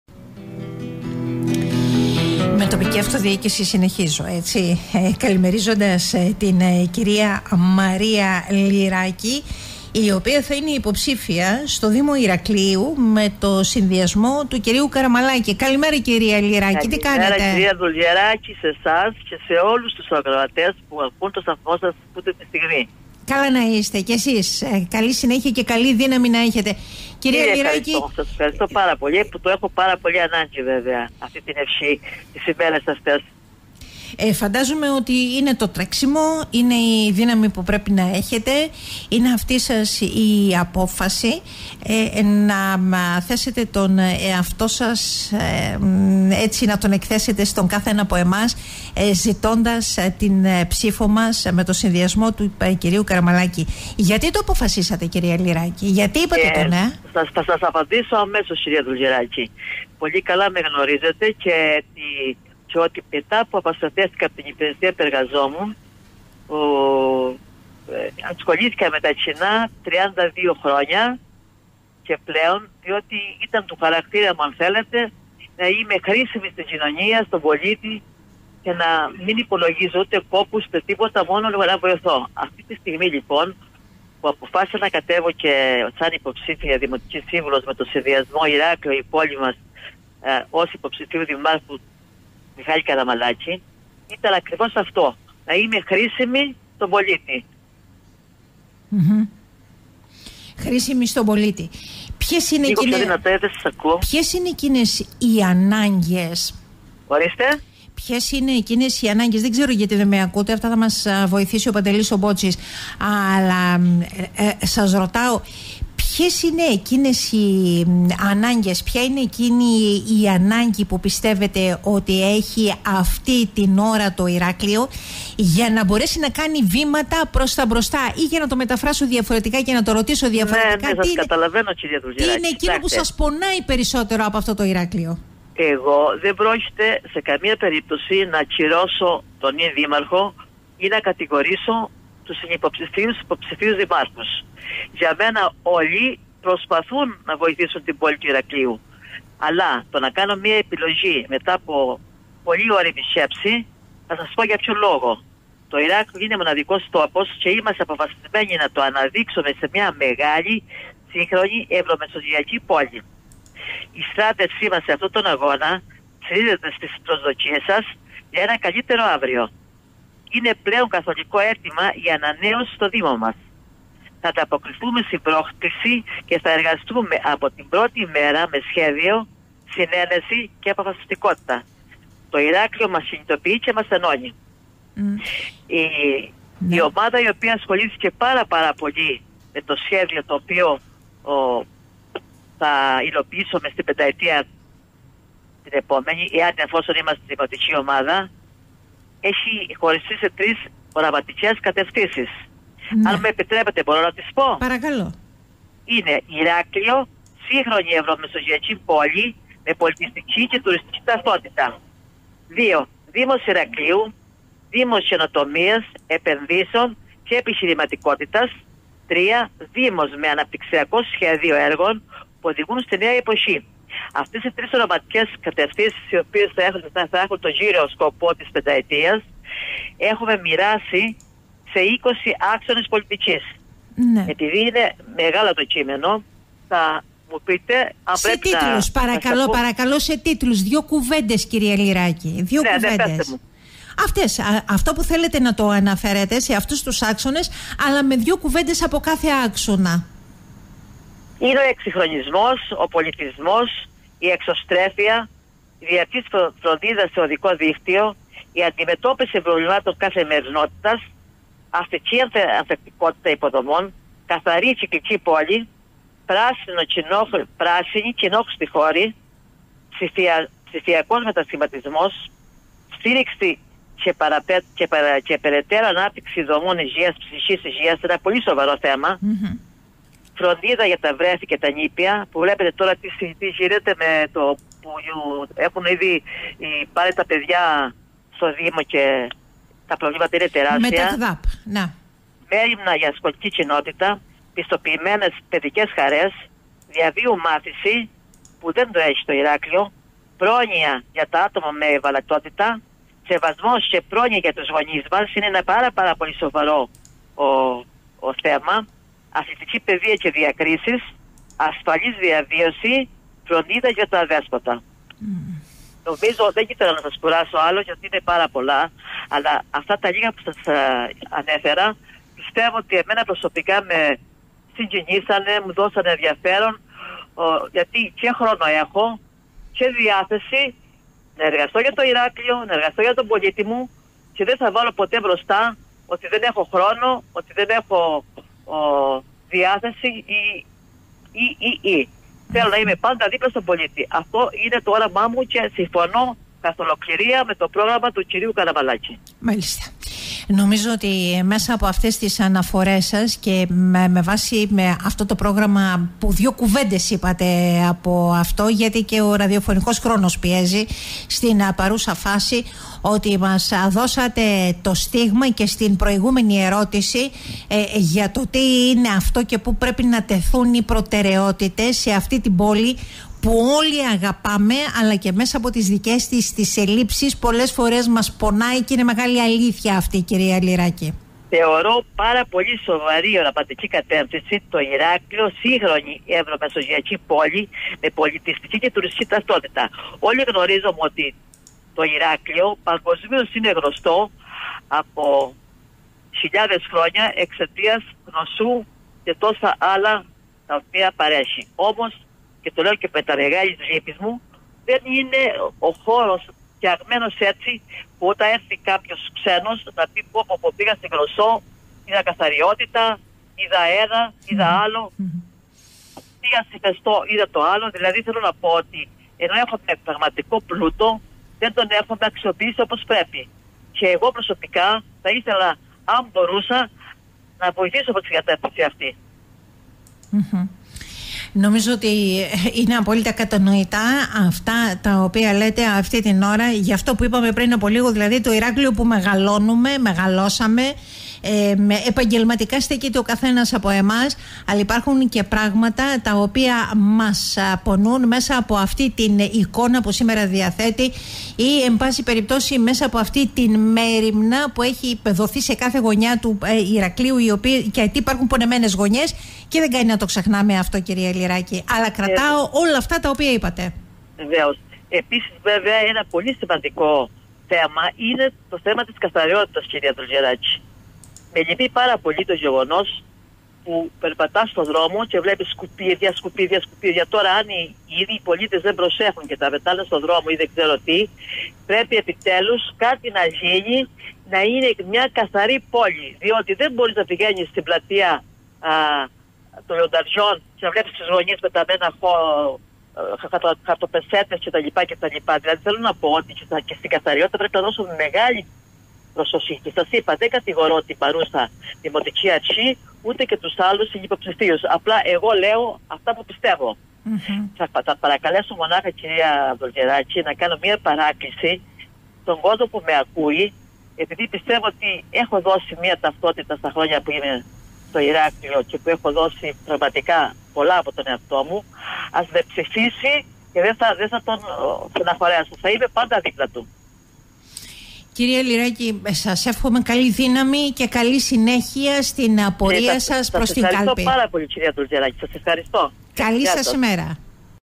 Μαρία Δογιάκη-Λυράκη: «Η προσφορά τμήμα της ζωής μου»
Υποψήφια με το συνδυασμό του Μιχάλη Καραμαλάκη στο Δήμο Ηρακλείου και με προσφορά 32 χρόνων στα κοινά, η Μαρία Δογιάκη-Λυράκη, μιλώντας στον 98.4 αναφέρεται στους άξονες του προγράμματος της παράταξης «Ηράκλειο – Η πόλη μας», που είναι το Ηράκλειο να αποτελεί μια σύγχρονη Ευρωμεσογειακή πόλη με πολιτιστική και τουριστική ταυτότητα, να είναι Δήμος Καινοτομίας, Επενδύσεων και Επιχειρηματικότητας και Δήμος με Αναπτυξιακό Σχέδιο Έργων που οδηγούν στη νέα εποχή.